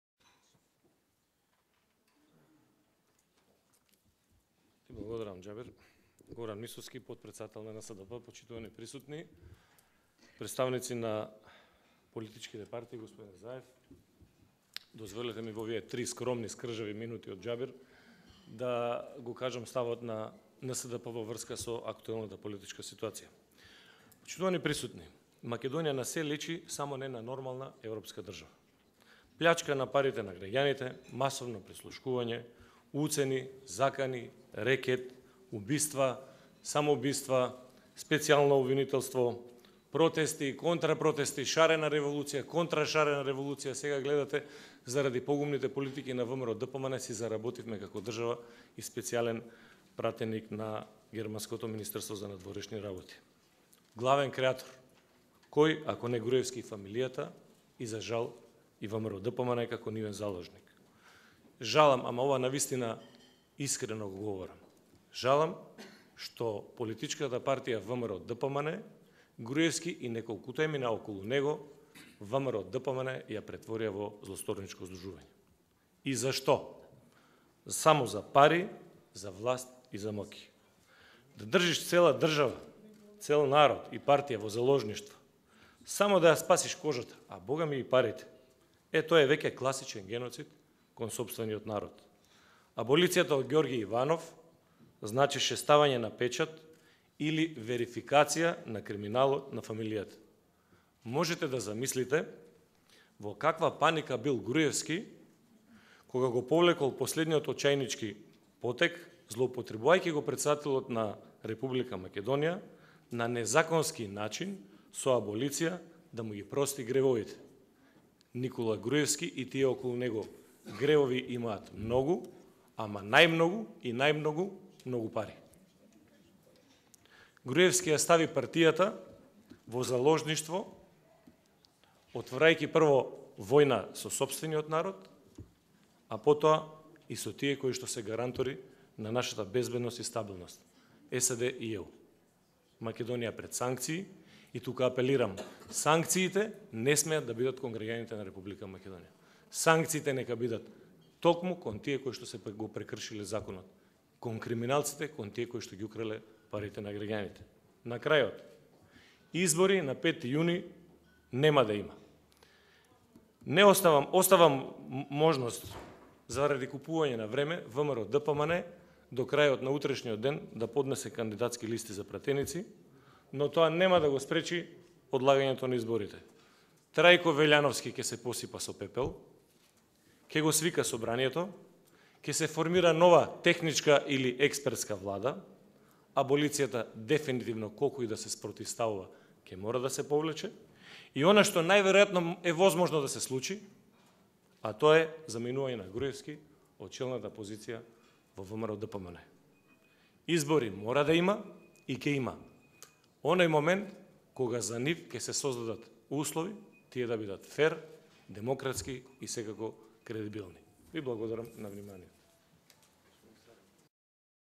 Мисовски: Аболицијата на Иванов е верификација на криминалот
Горан Мисовски, потпретседателот на НСДП ги претстави позициите на својата партија на дискусијата „Плурализмот и политичката криза“ во организација на ЦИВИЛ – Центар за слобода. Неговиот став е дека нема услови за слободни, фер и демократски избори на 5-ти јуни.
„Груевски и фамилијата ја претворија ВМРО-ДПМНЕ во злосторничко здружување. Избори на 5 јуни нема да има. Вељановски ќе се посипа со пепел и ќе го свика повторно Собранието на Р.Македонија. Избори мора да има и ќе има, кога ќе се создадат услови. Аболицијата на Иванов е ставање на печат и верификација на криминалот. Замислете во каква паника бил Груевски, кога го повлекол последниот очајнички потег, користејќи го претседателот на државата”, заклучи Мисовски.

Настанот се одржа во рамките на проектите „Слободни избори за слободни граѓанки и граѓани“, поддржан од Министерство за надворешни работи на Сојузна Република Германија и „Напред!“ – проект за изборен мониторинг, финансиран со германски средства од Пактот за стабилност на Југоисточна Европа.